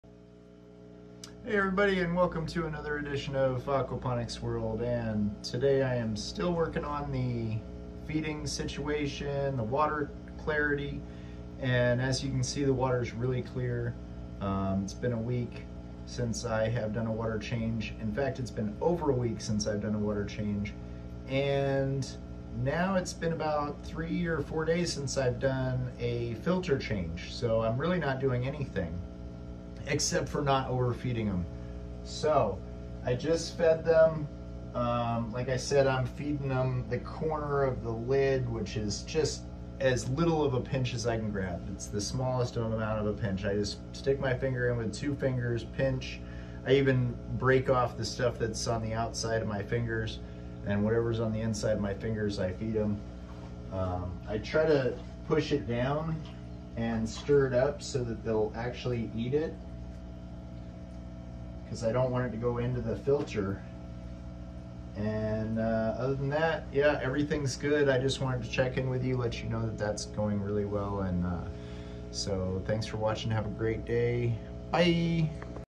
A pinch of food no more is really the secret video 94
In this video, we reveal the secret to feeding your aquaponic fish for optimal health and ecosystem balance. Learn why 'a pinch of food and no more' is the golden rule for preventing overfeeding disasters and achieving thriving, happy fish. With expert tips and vibrant visuals, your aquaponics setup will never be the same!